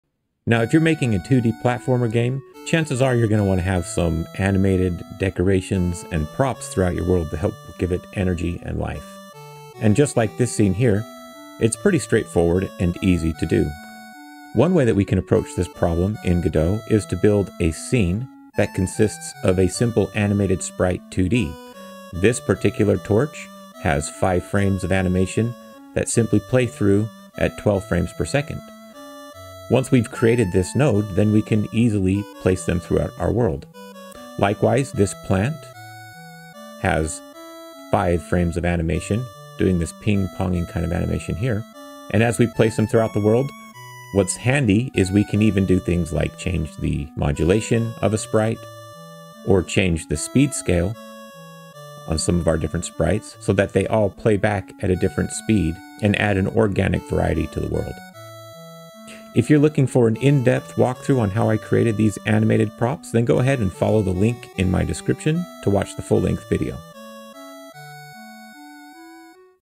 Turn DEAD levels into ALIVE ones in 60 sec!
Watch these plants & torches come to life with AnimatedSprite2D + reusable scenes ⚡
Bonus: animated tiles & quick performance check!